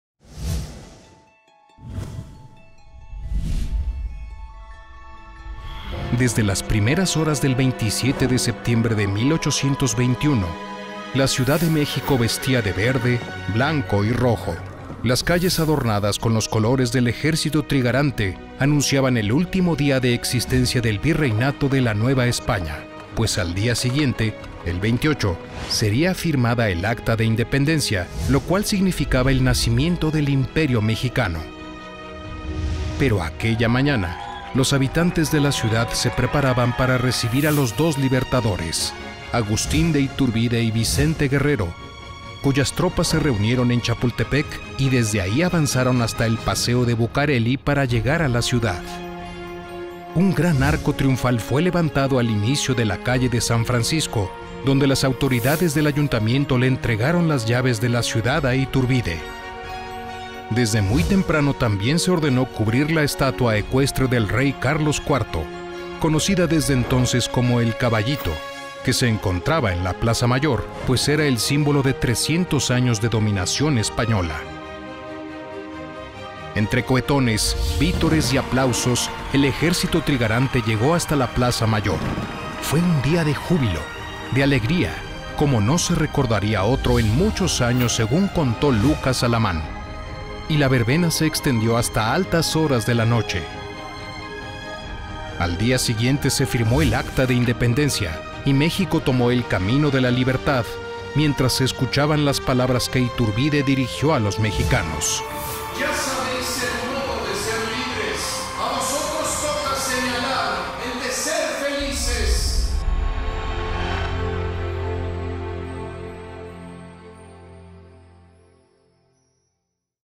Voces de otra época. La consumación. Ep. 4
A través de estas cápsulas animadas, se narra la consumación de la guerra de Independencia de México, desde las estrategias que los protagonistas llevaron a cabo, hasta las alianzas que formaron Agustín de Iturbide y Vicente Guerrero.